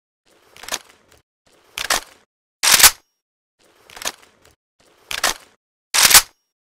MK18 animations [rusty]
i animated it in 60fps but [thanks camtasia] its in 30 fps

sounds by navaro i am allowed to use them ya ok ya

i also havnt animated FPS in a while so ... rusty

yea. OH YEA IM GONNA MAKE A INVERTED TRAILER ONC EI FIX THE PROBLEMS !